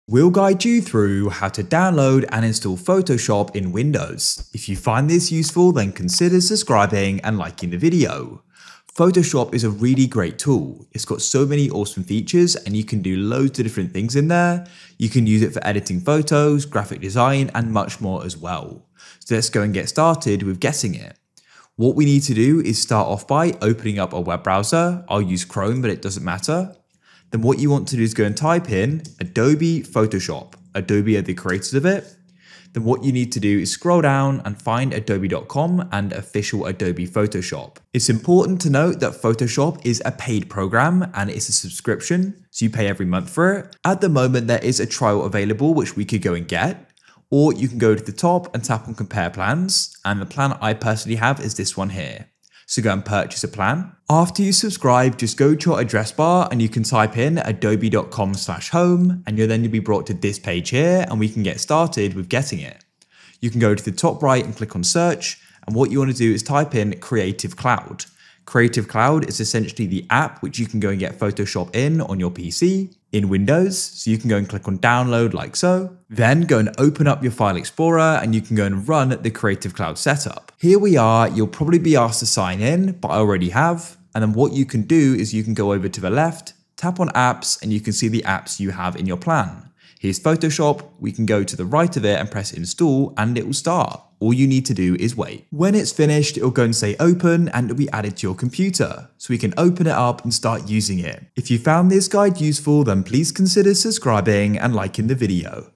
How To Download Photoshop On Laptop & PC - Full Guide
Learn how to download and install photoshop on laptop and pc windows in this video.

GuideRealm